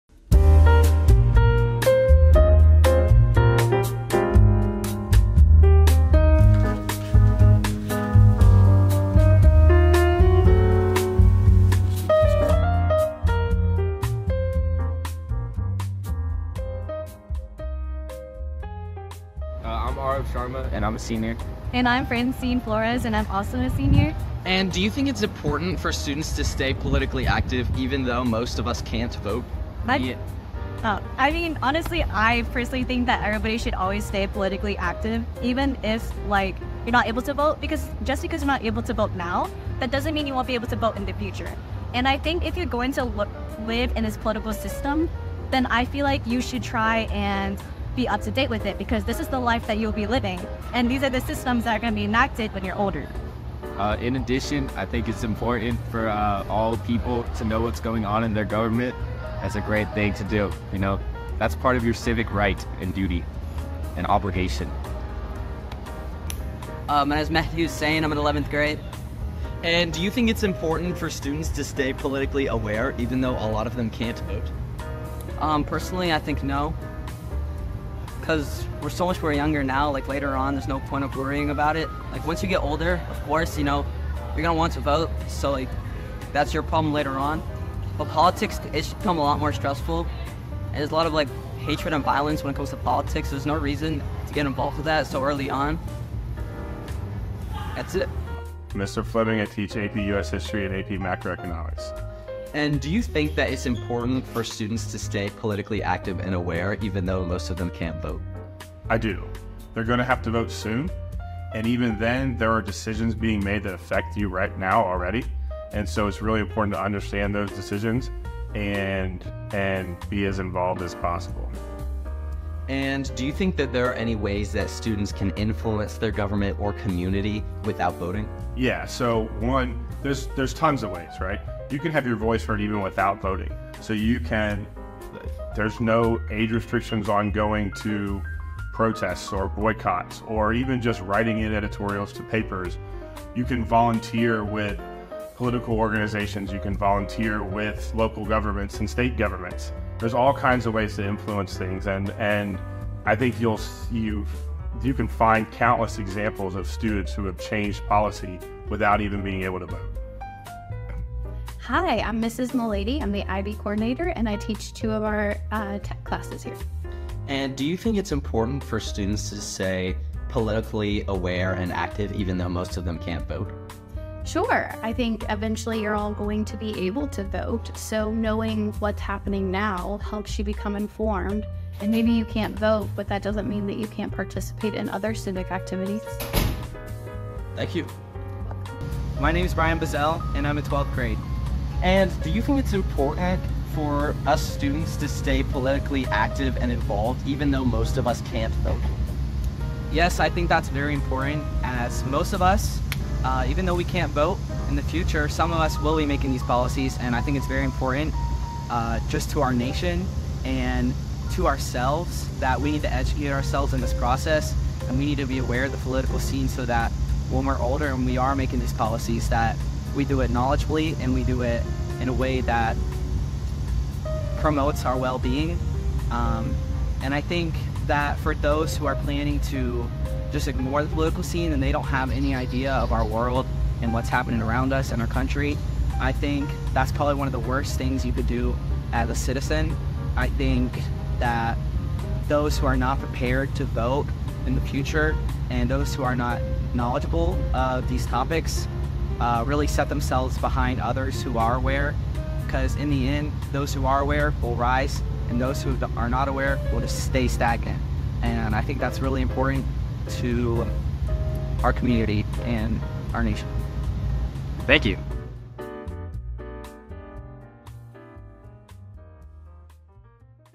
The 2024 U.S. Election Video
In this video, students and faculty at Stanton College Prep discuss the importance of political activism and involvement for young people.

Interviewer: Rowan Kershner
Videography: Arvind Mukund
Editing: Arvind Mukund